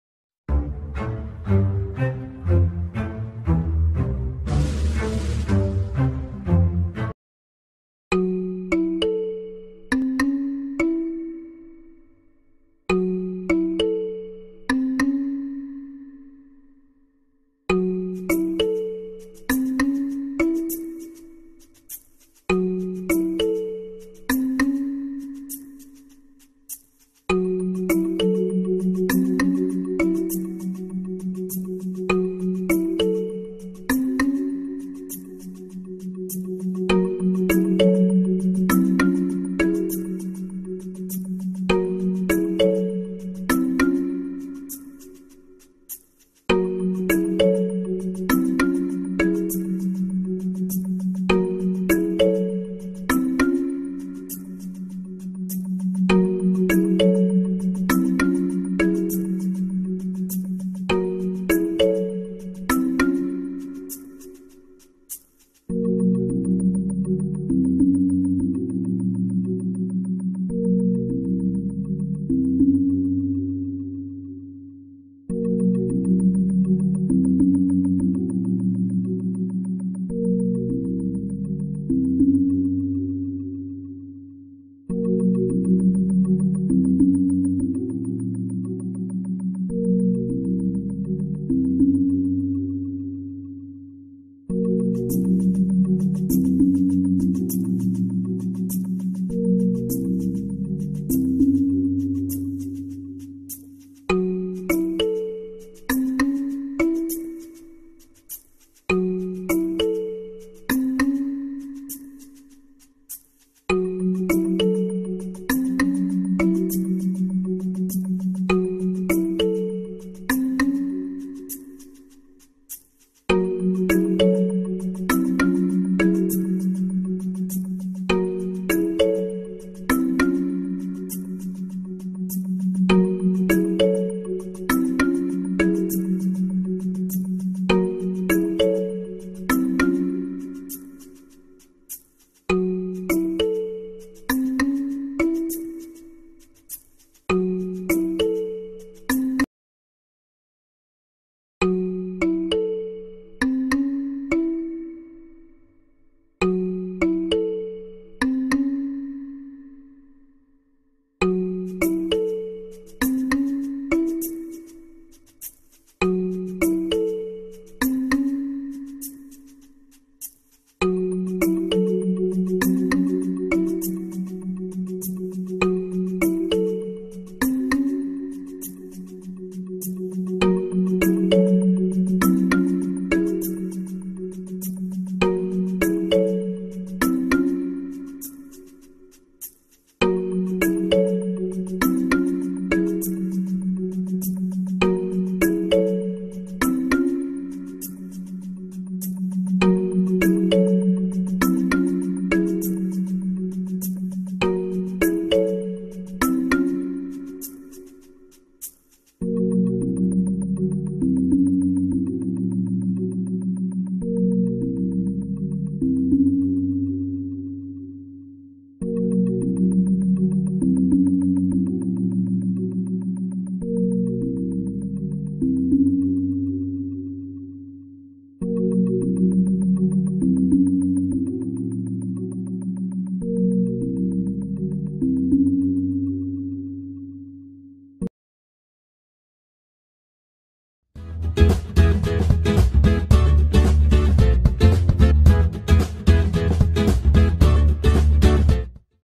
DIY/best out of waste/reusing old dolls/reuse ideas/kids clip organizer/hair clip organiser crafts
This video is about the making of easy clip organizer by things available at home. This video helps to make use of readily available old things from home. it also helps to find the hair clips easily in using it with our busy day to day schedule. actually its one of our creativity during the lockdown horror.